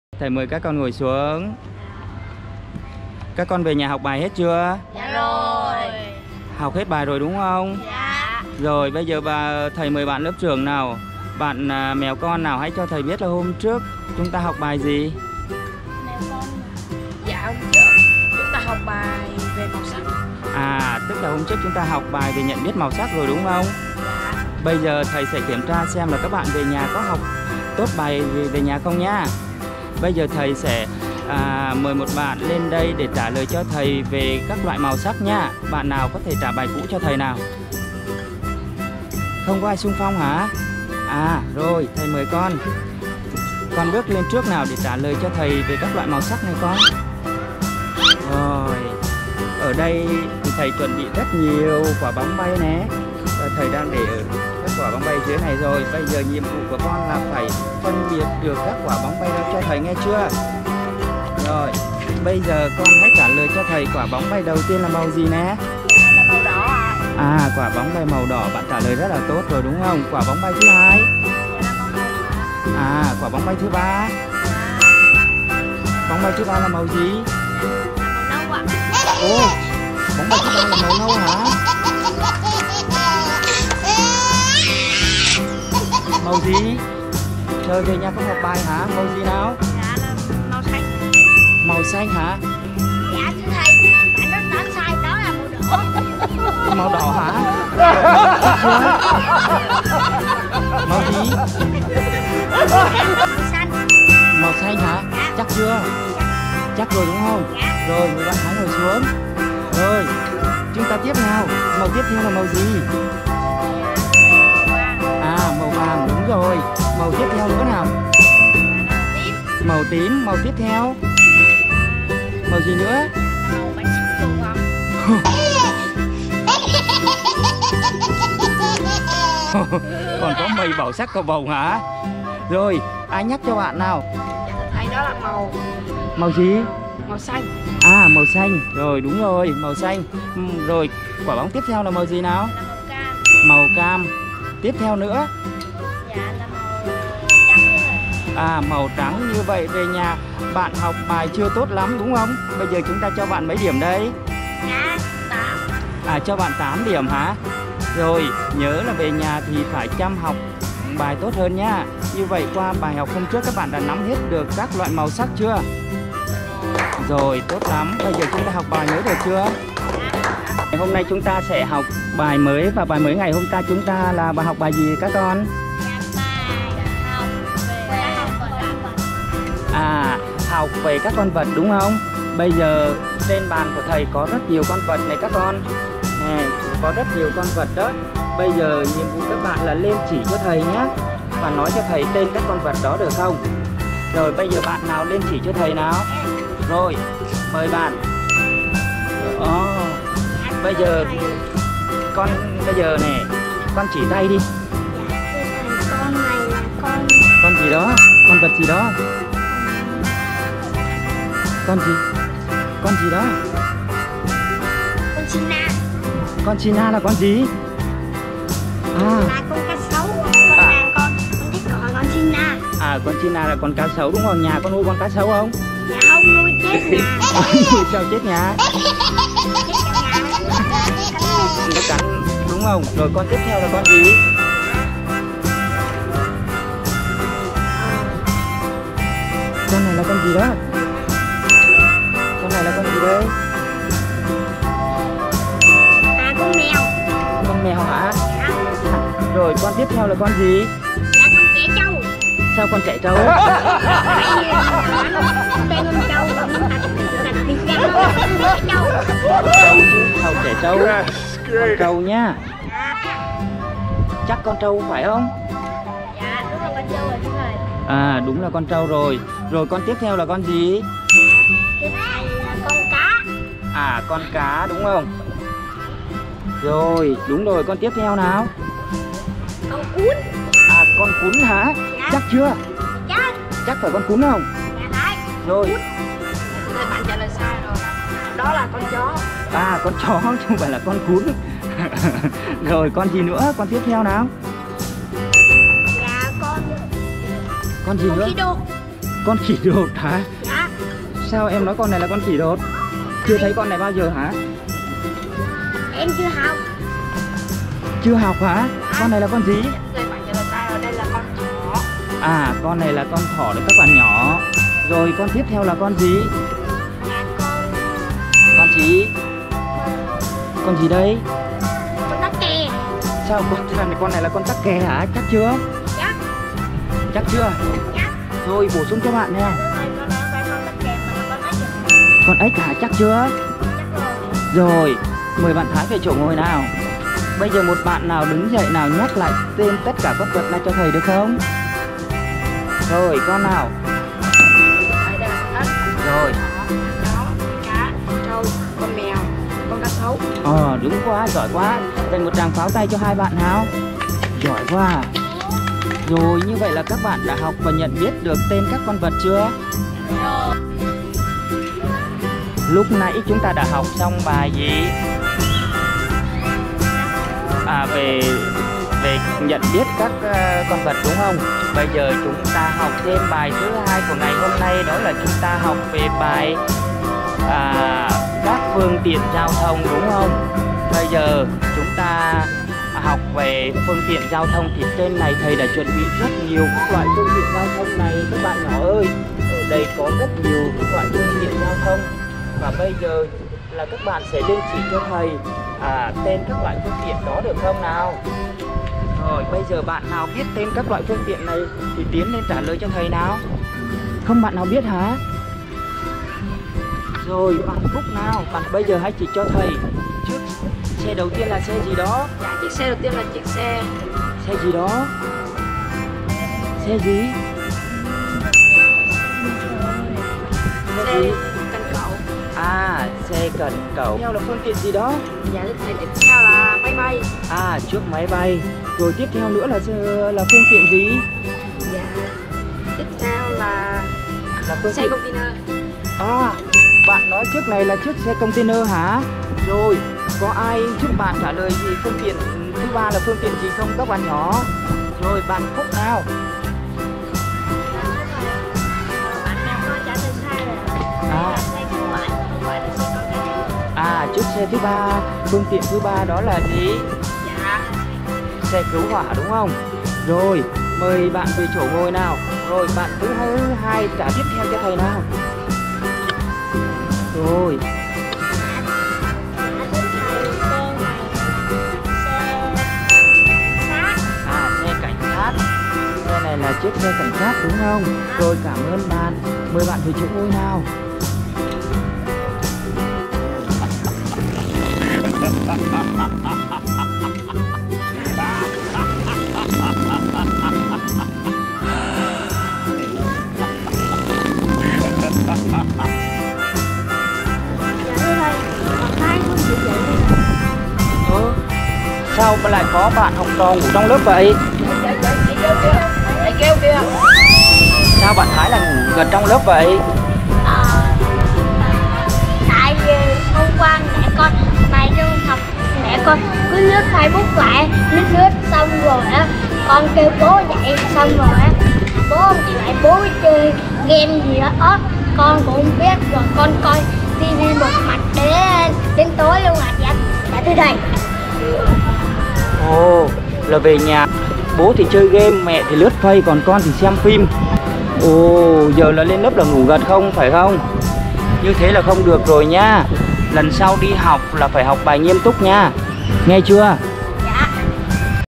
Trò Chơi Giúp Bé Biết Phân Biệt Màu Sắc, Tên Các Con Vật Và Các Phương Tiện Giao Thông!
Trò Chơi Giúp Bé Thông Minh Biết Phân Biệt Màu Sắc, Tên Các Con Vật Và Các Phương Tiện Giao Thông. Trò chơi giúp các bé hình thành dức tính tự tin, hiểu biết của mình và khả năng phán đoán sự vật. đảm bảo sau video các bé sẽ thuộc tất cả những gì mà video đã trình bày. Mong mọi người ủng hộ vì tính nhân văn của video! Xin Chân Thành Cảm Ơn!
Nếu thích video này bạn đừng quên nhấn vào [ĐĂNG KÝ] + [NHẤN CHUÔNG] để ủng hộ kênh cho mình nhé!!! Cảm ơn các bạn nhiều!